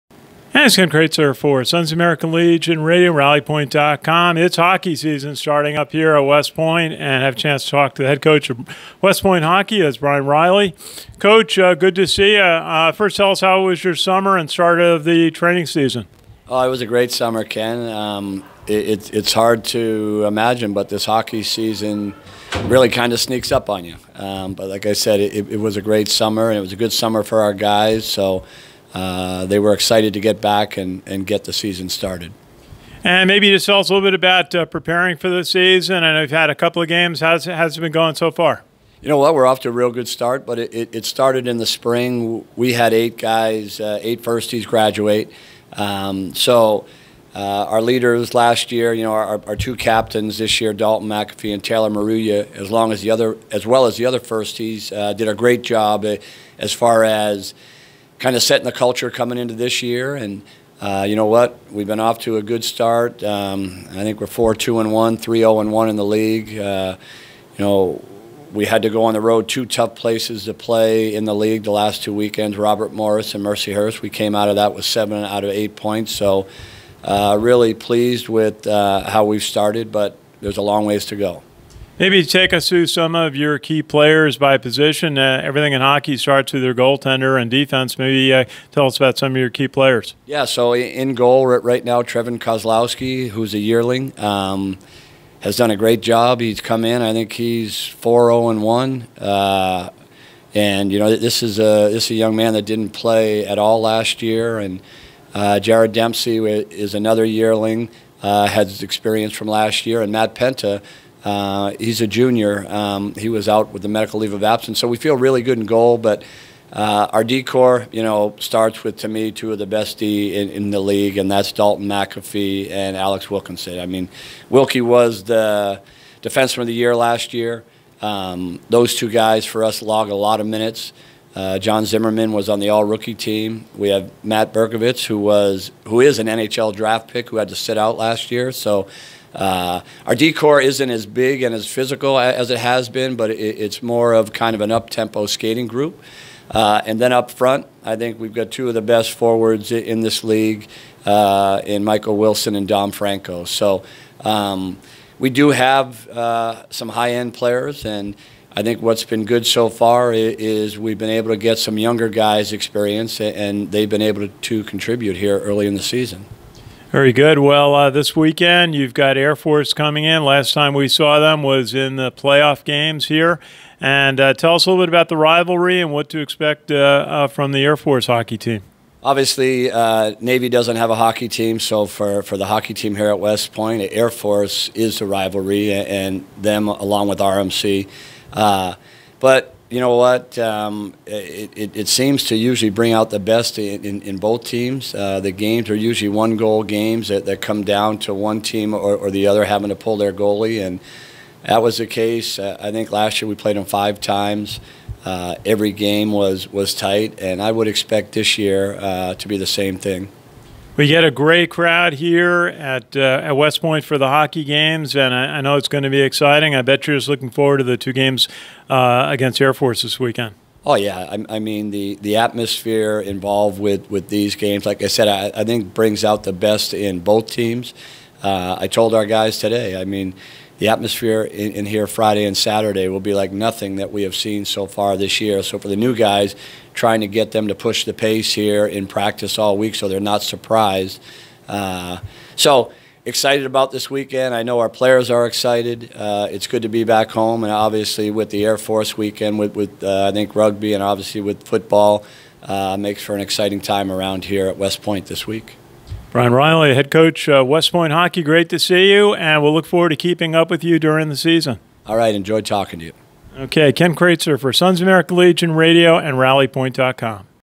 Talking Army West Point Hockey with Coach Brian Riley
Video by Ken Kraetzer for SALRadio and Rallypoint.com on 20181030